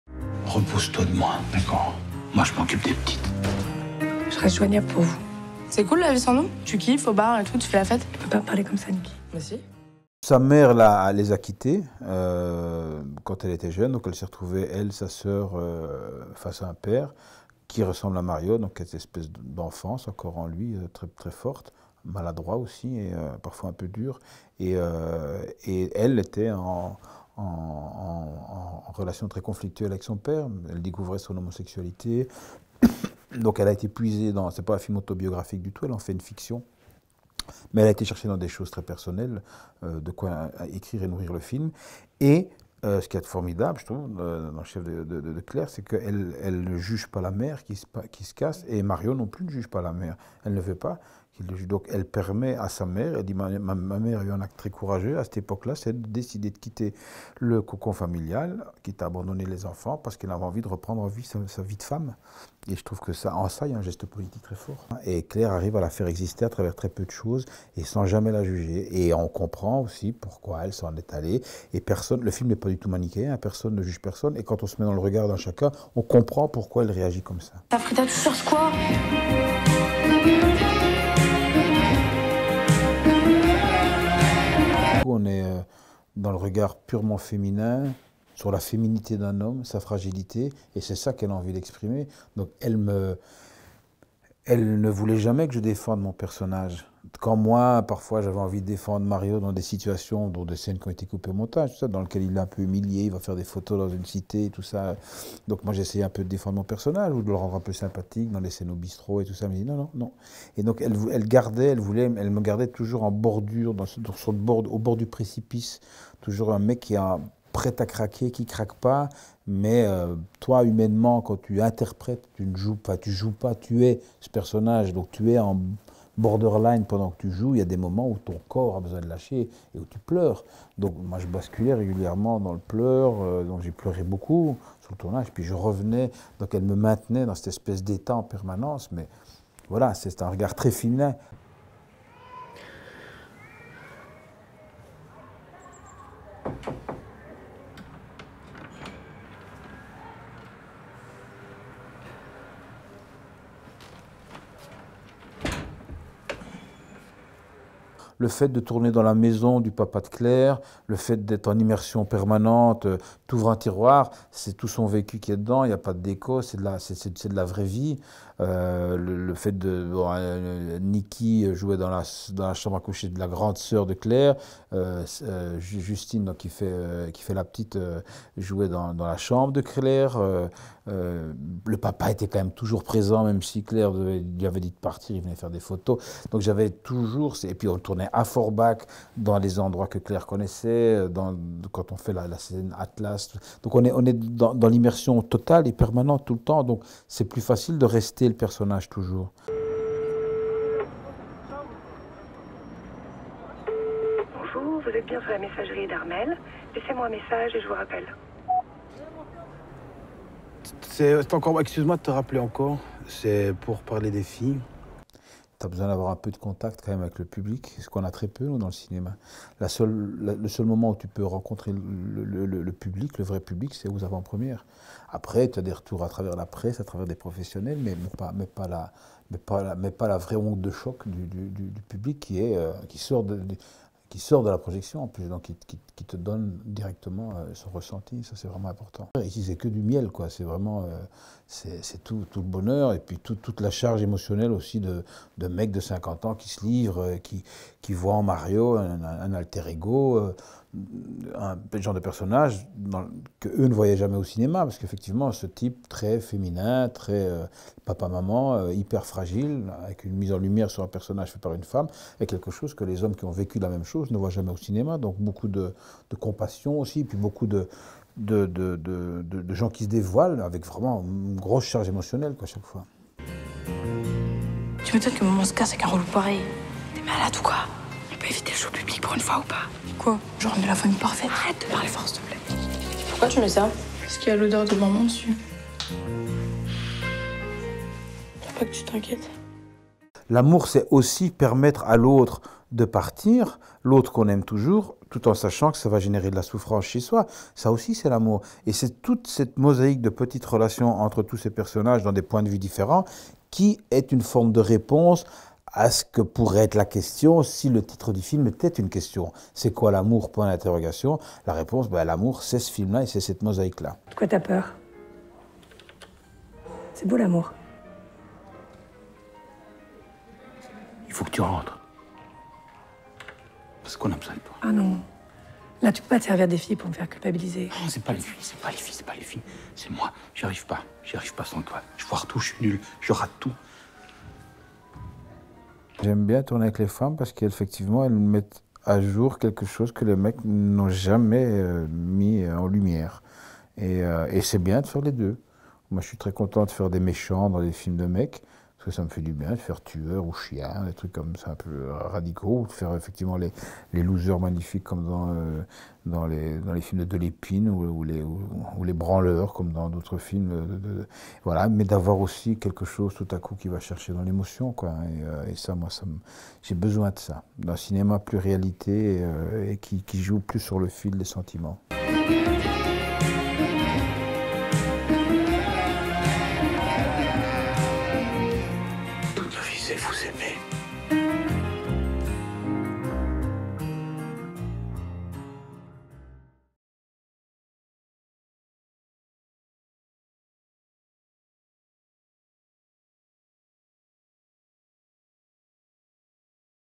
Bouli Lanners: C'est çà l'amour
Quand Armelle décide de prendre de la distance, Mario se retrouve avec ses deux filles dans la maison familiale. L’aînée va fêter ses 18 ans, la cadette découvre l’amour et cela ne va pas de soi. Mario, lui, tente de tenir la barque, de rassurer ses filles, de retrouver sa femme, qui le quitte, pourtant, inexorablement. Prix du meilleur réalisateur aux Journées des Auteurs du Festival de Venise cette année, C’est ça l’amour ne paie pas de mine et c’est là toute sa saveur. Humble et délicat, le film est le portrait gracieux et sensible d’un homme blessé dans la tempête d’une séparation.